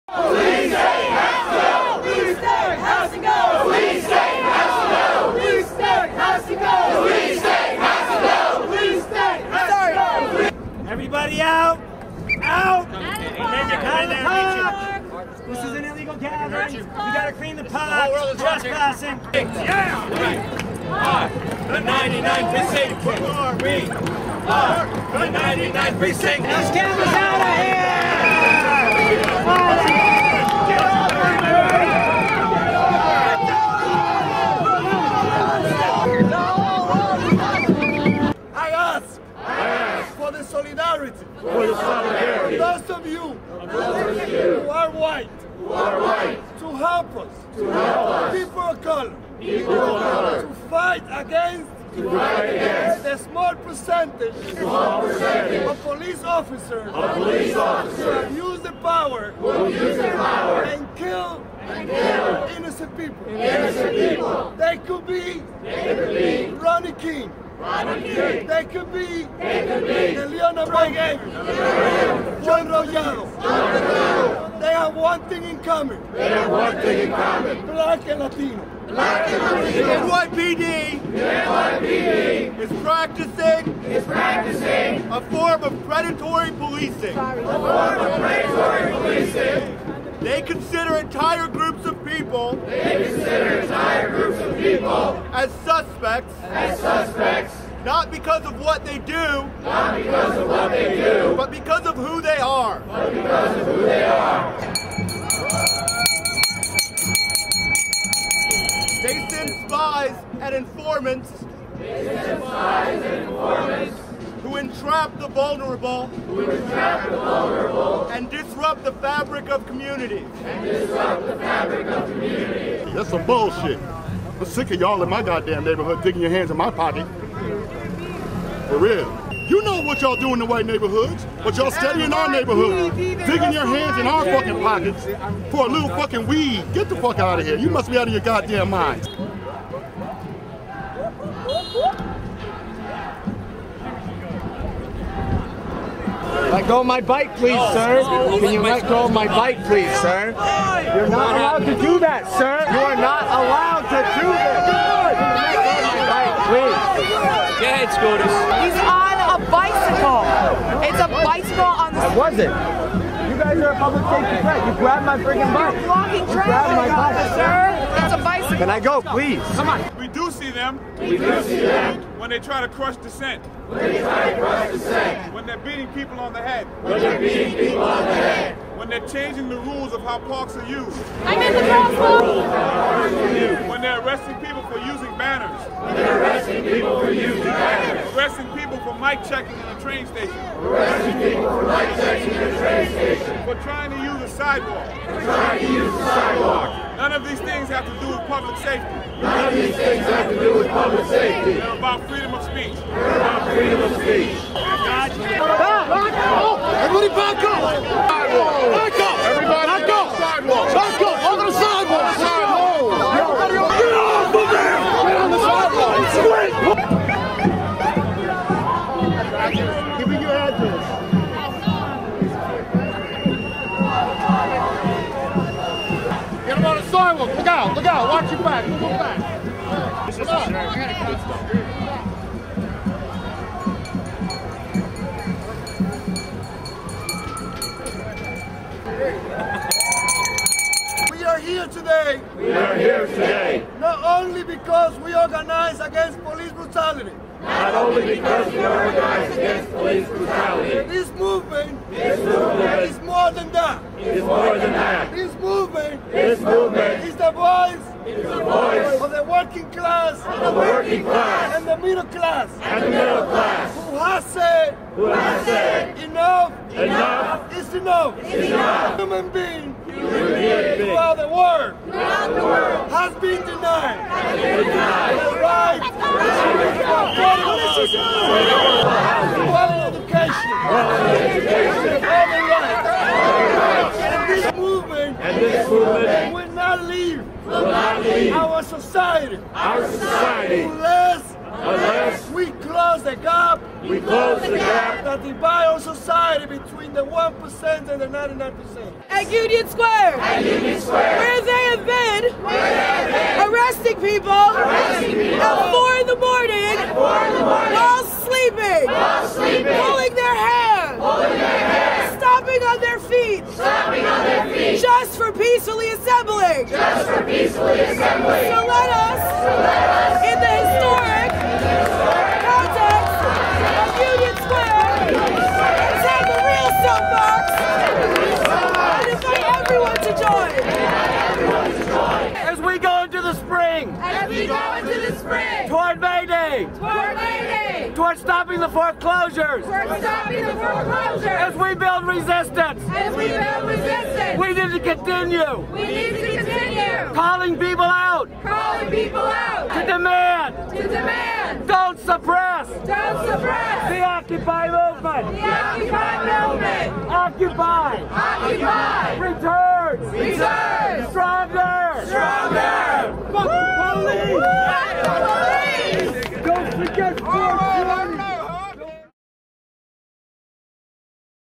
M24 March Against Police Brutality
NYC, March 24: Occupy Wall Street stages a protest against the suppression of the movement, and of police brutality, surveillance and infiltration. Demand are made for the resignation of Police Commissioner Ray Kelley and justice for slain teenager Trayvon Martin. 600 people rally in Zuccotti Park and march to Union Square, at times taking the streets. The NYPD responds with a huge presence, including mounted police and squadrons of motor scooters. At first they seem to avoid making arrests, then swiftly change tactics, sending in squads equipped with plastic handcuffs, and charging into the crowd to make arrests.